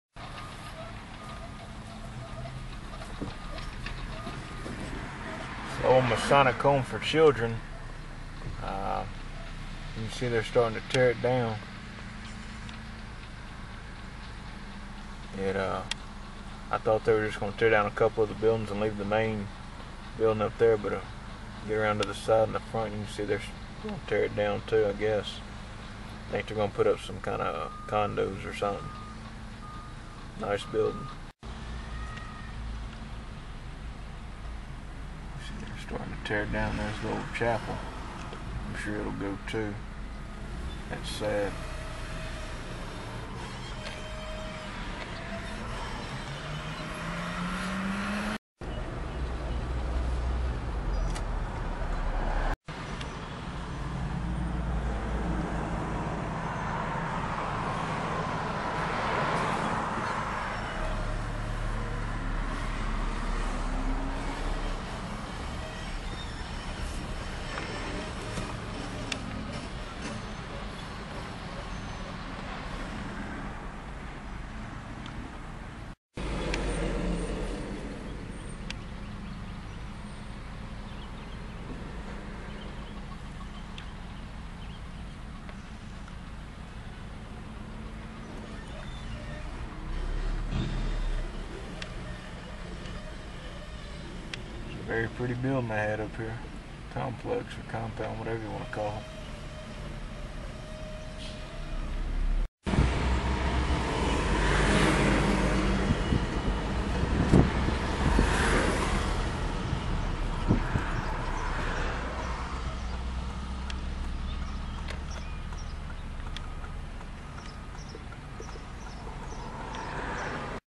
Masonic Home for Children in Alexandria, LA being torn down!
This is a little video of them tearing down the majority of the Masonic Home for Children.  This was built in either 1924 or 1925 by the Masons as an ophanage, and I believe it operated until the late 80's or maybe early 1990.I can remember riding with my aunt at a young age and telling her about the home, and she said she had recently sent them money.  I believe that I read somewhere that it cost $250,000 to build back then.    Anyway, I always though the buildings were beautiful, and would have loved to have seen the inside.  I heard it was on the national historic register, and I always thought if a building was on that register, that you couldn't alter it.  But then again, how can they tell you what to do with your property.  I believe they are going to put some apartments or condos here, and I think they are gonna use part of the main building.  I thought there was a Louisisana Historical Marker in the front of the property, and wanted to film that, but I guess they took it down before I got around to it.  Just thought I would share this.  If you have any facts about this building, please leave a message.  Thanks!